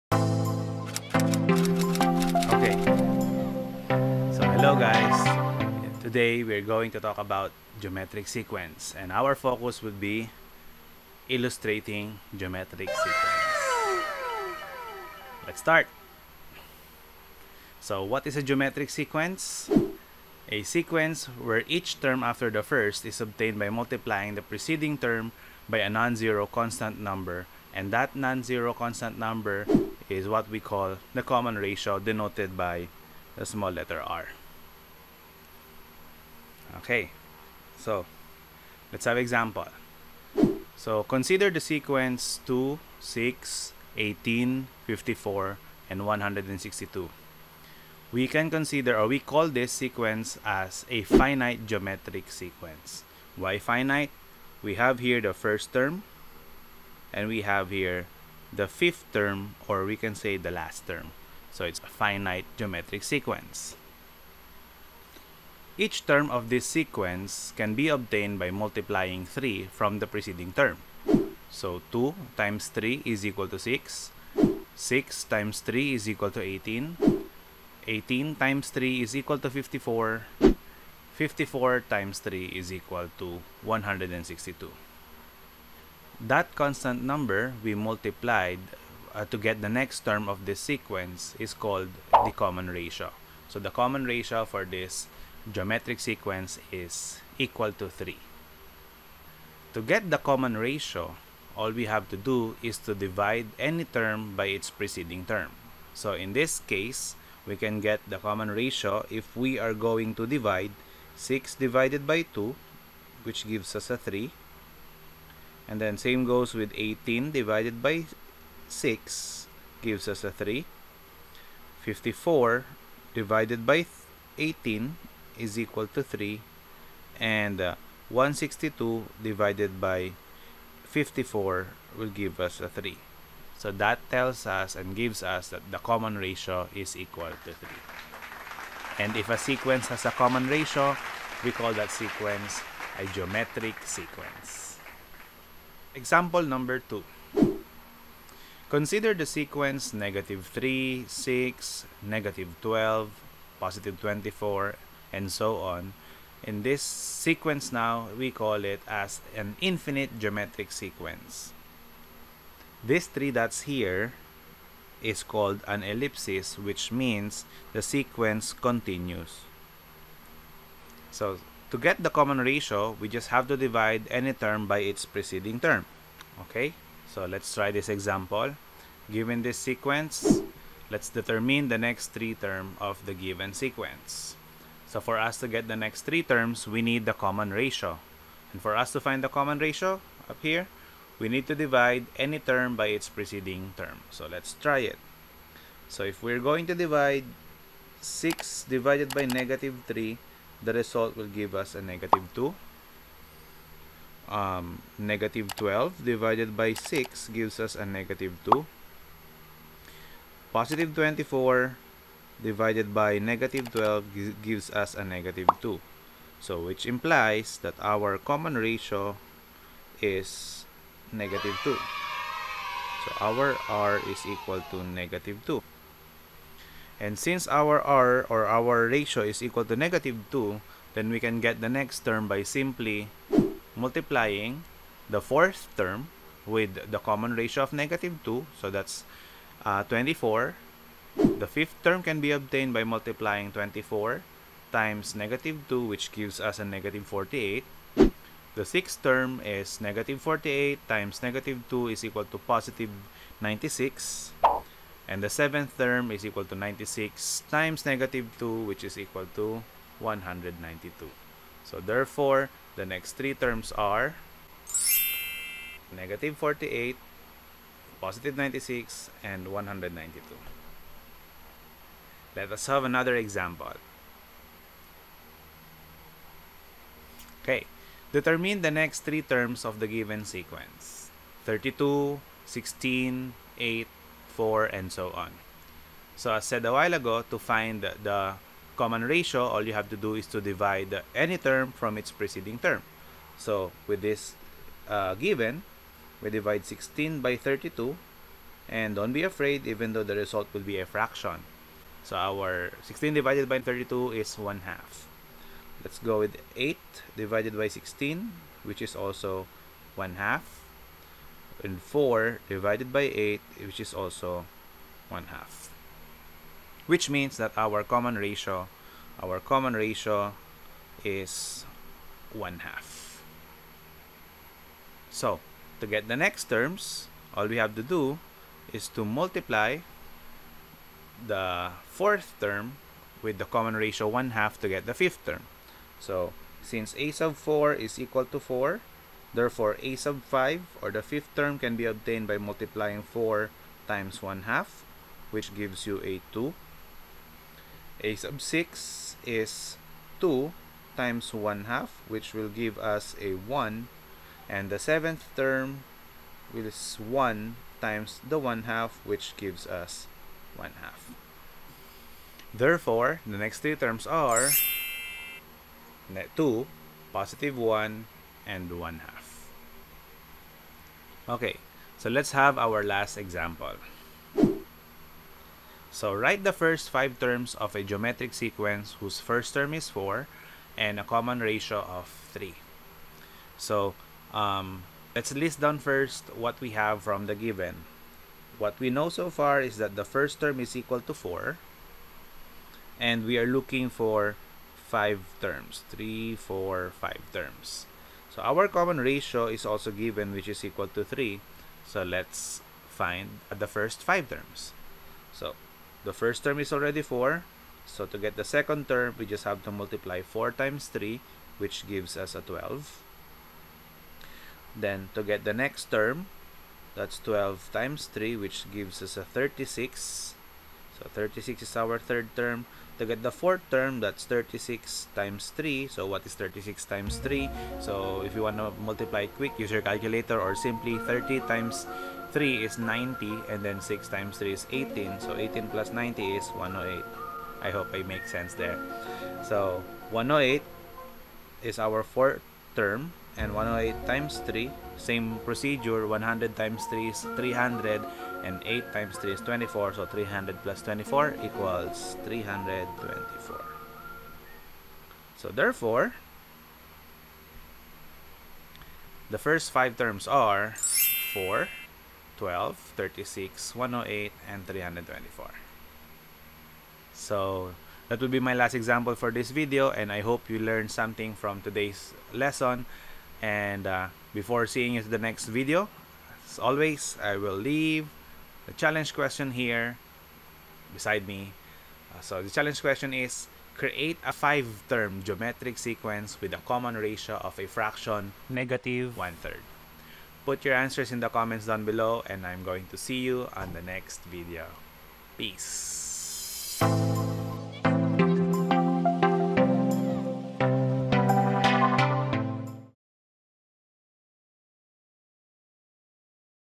Geometric Sequence - Illustrating Geometric sequence
Geometric Sequences: illustrating geometric sequence

In this video, examples were used to know how to find the common ratio of a given geometric sequence and also how to illustrate geometric sequence

challenge question at the end of the video

Hope you learn even a little from this video.